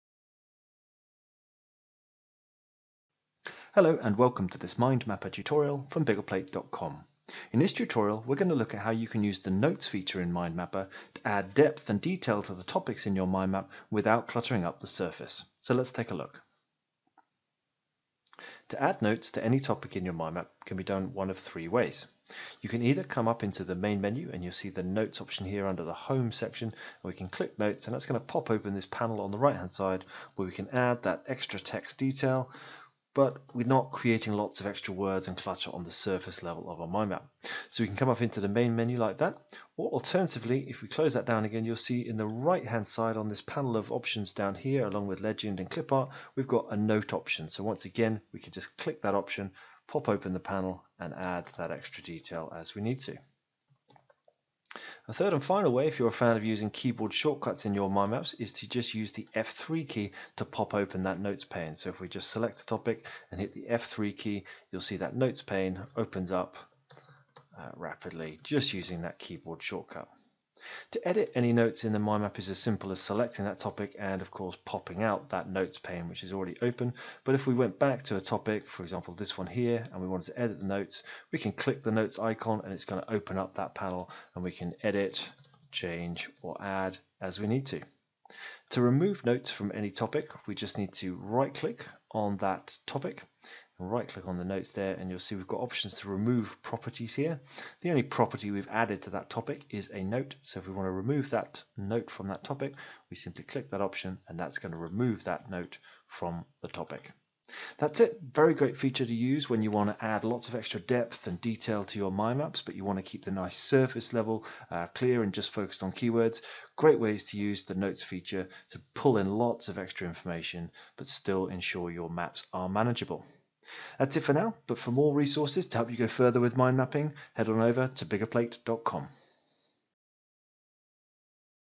MindMapper Tutorial - Topic Notes
In this tutorial we show you how to use the Notes feature in MindMapper to add extra depth and detail to your mind maps, without creating clutter in the mind map.

For more resources to help you go further with mind mapping,

Found this video useful? Why not buy us a coffee!